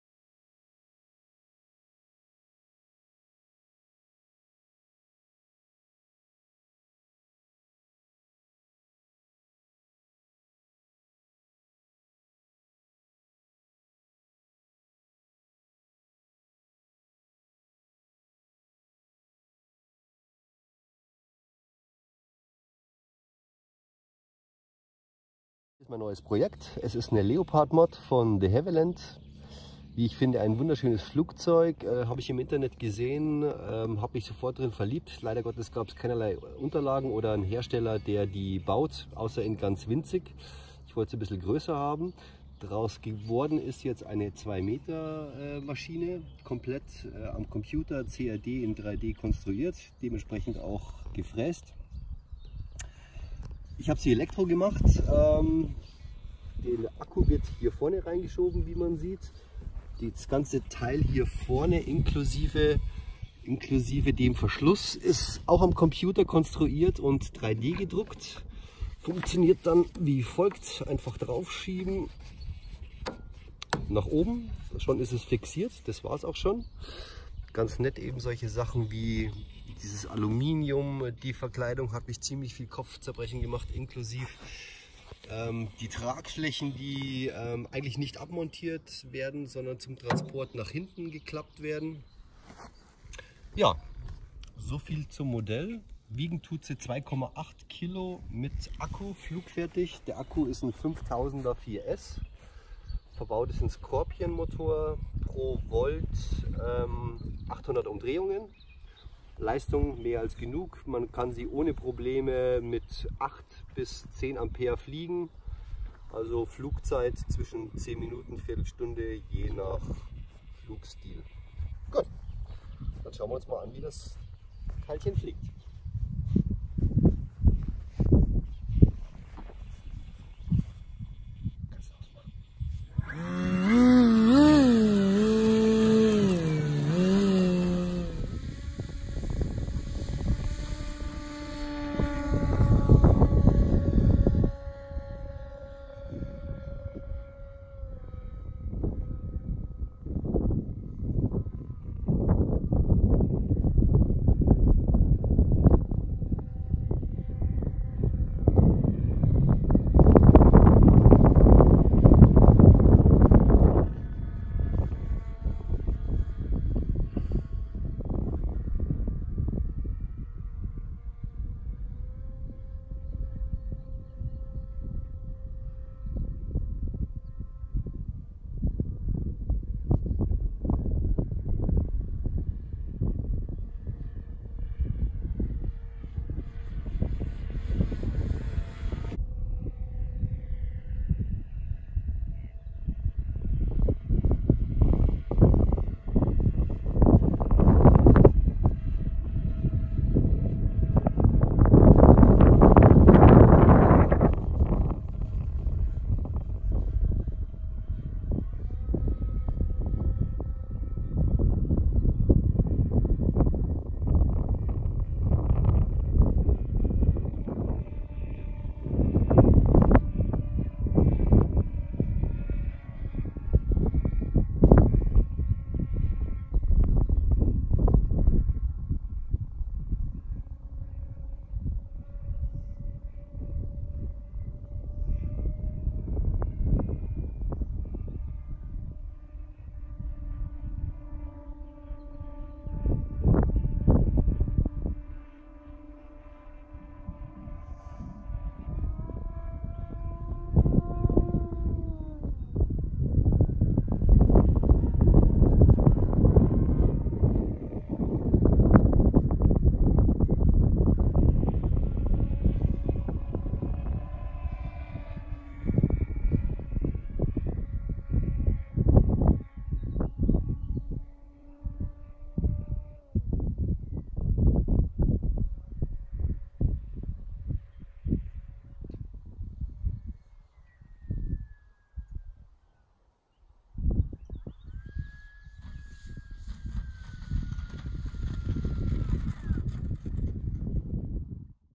Leopard Moth von de Havilland - Michael Robin
Vorstellung der Leopard Moth (De Havilland) als Modell mit ca. 2m Spannweite. Konstruiert und gebaut von Michael Robin. Ein neues Meisterstück aus der Monte Baldo Werft :-)